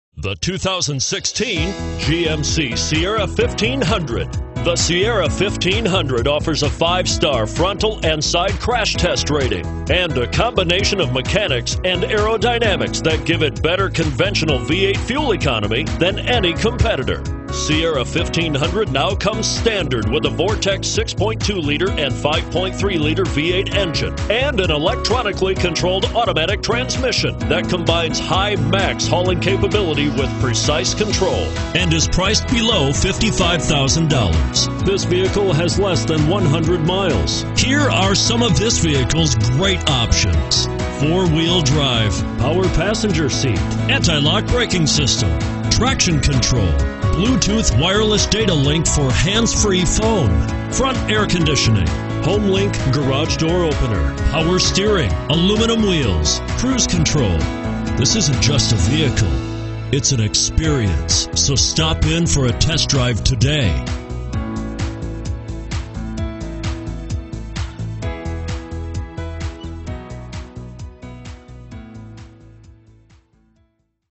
2016 GMC Sierra 1500 Minneapolis St. Cloud & Monticello MN G16-183
2016 GMC Sierra 1500 SLT

You'll love this 2016 GMC Sierra 1500. This is a truck you'll want to take home. With 10 miles, it features Automatic transmission and an exterior color of QUICKSILVER METALLIC. Call and get in touch with West Metro Buick GMC of Monticello directly, and be the first to open the truck door today!